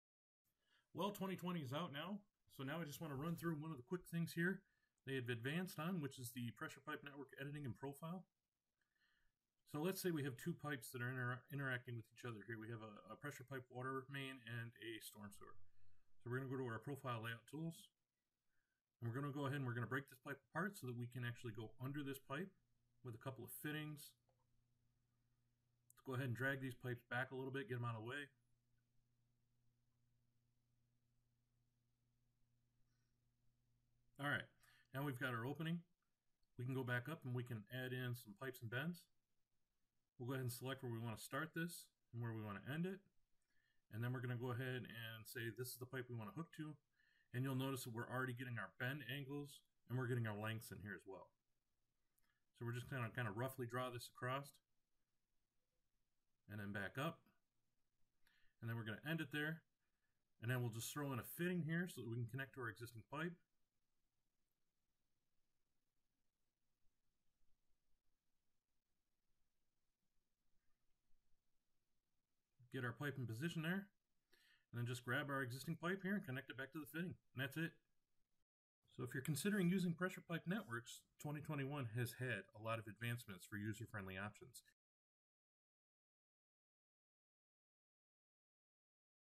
Civil 3D Pressure pipe profile editing
In 2021 Autodesk made significant advancements in the pressure pipe area of Civil 3d. Here i will break the pipe and re-route under a conflicting pipe using the profile editing tools.